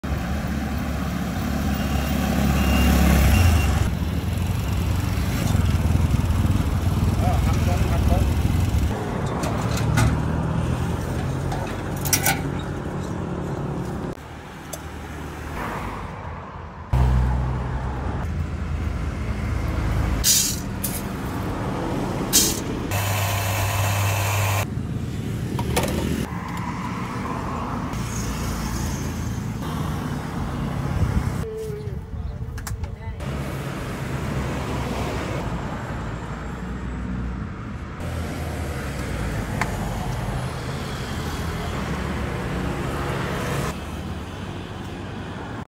Unique Coffee Bar on Weels Young Barista Start Small Business Ideas By Moka Pot
Unique Coffee Bar on Weels Young Barista Start Small Business Ideas By Moka Pot - Thai Street Food